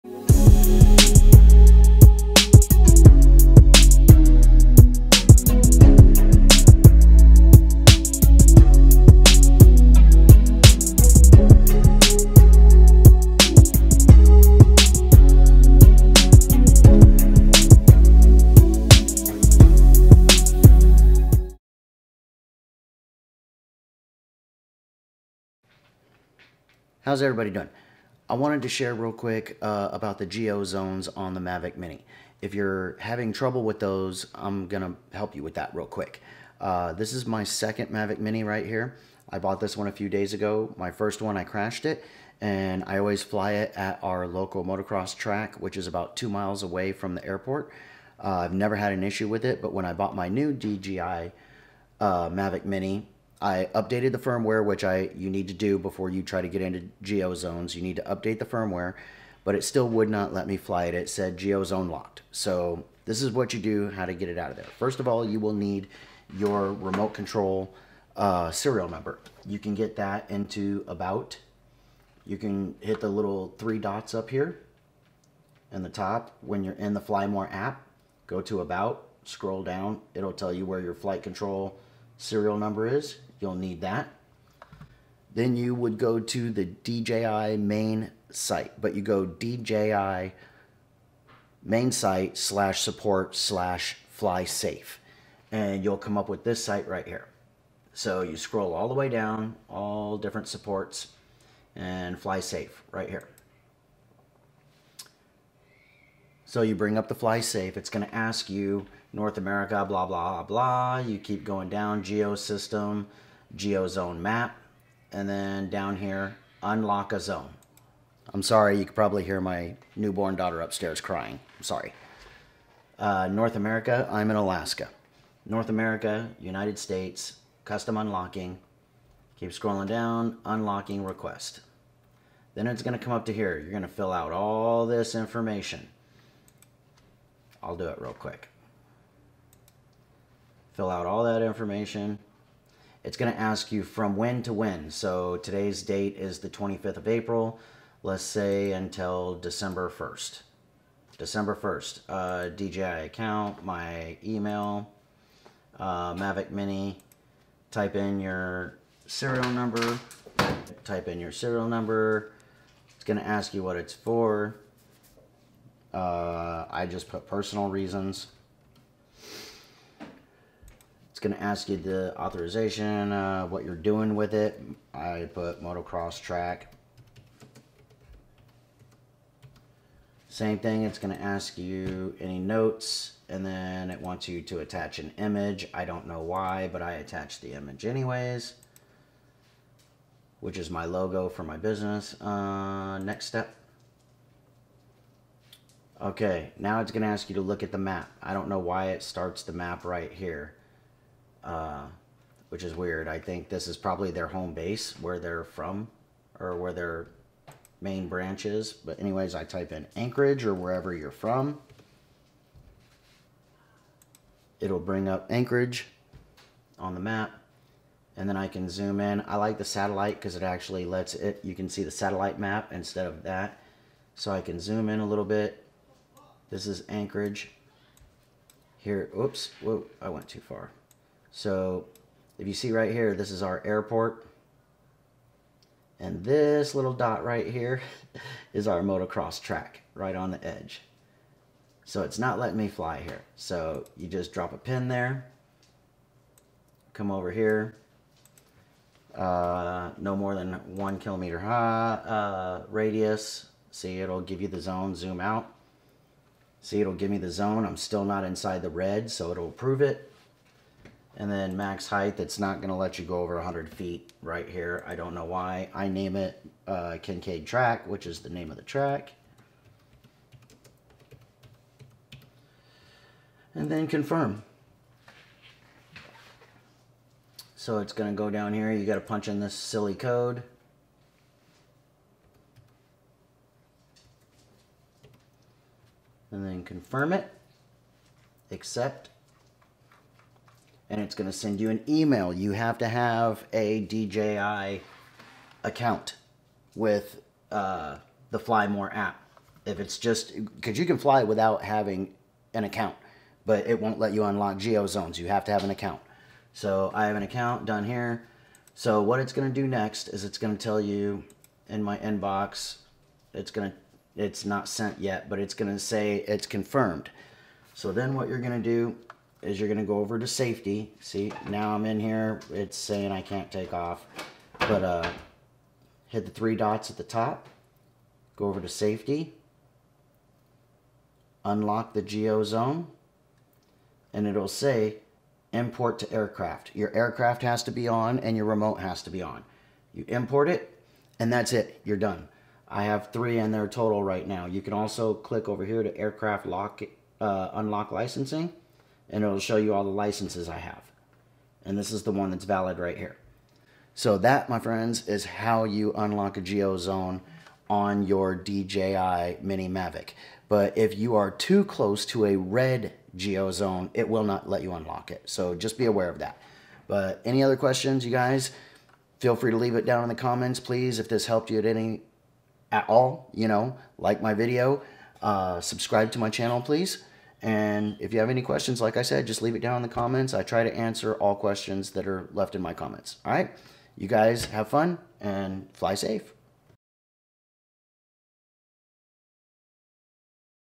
Mavic Mini | Unlocking Geo Zones
Having trouble getting your Mavic Mini to fly in a Geo Zone? Let me help!!